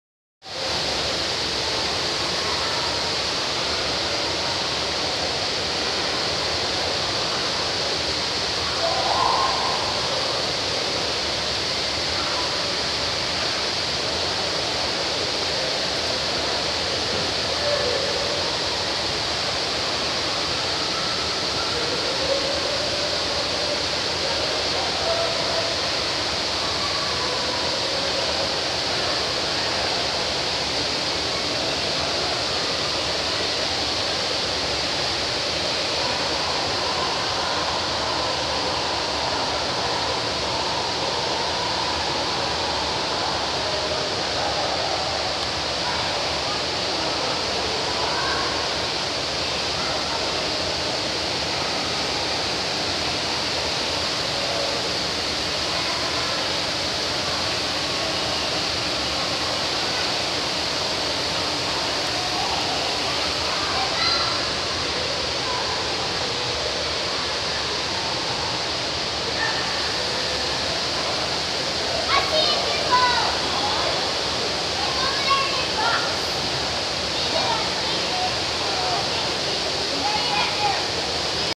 River Otters in the Lied Jungle at Henry Doorly Zoo
River Otters in the Lied Jungle at Henry Doorly Zoo in Omaha Nebraska